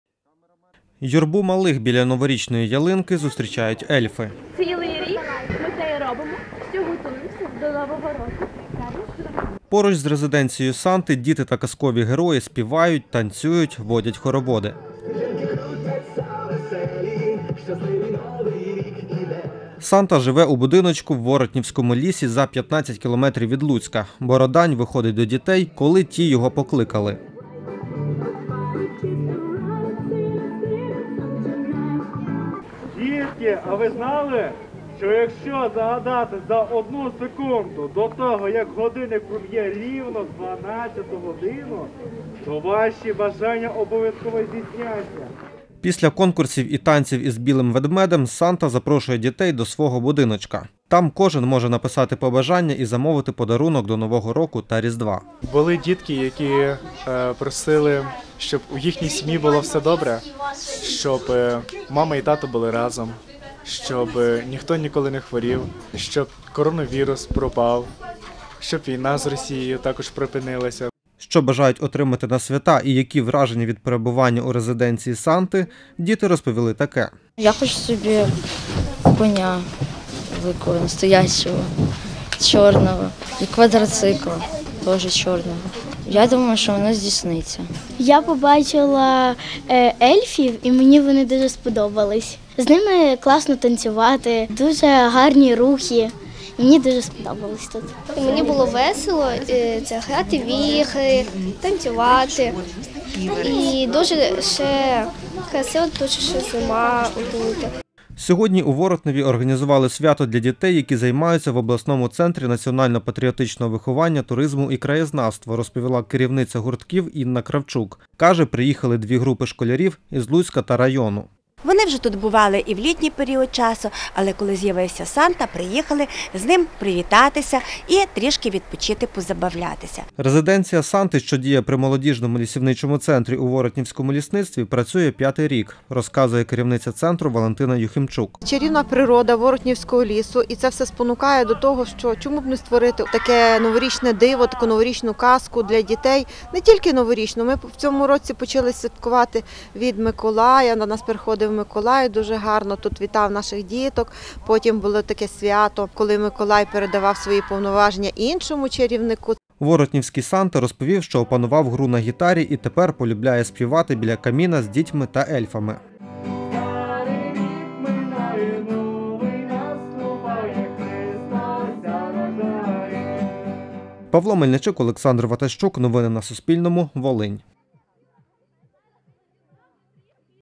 Як працює зимова резиденція біля Луцька
У Воротнівському лісництві в Луцькому районі запрацювала резиденція Санти. Оселився святий у будиночку лісівничого молодіжного центру за 15 кілометрів від Луцька. Як розповіла Суспільному керівниця лісівничого молодіжного центру Валентина Юхимчук, резиденція Санти, що діє при молодіжному лісівничому центрі у Воротнівському лісництві, працює п'ятий рік поспіль.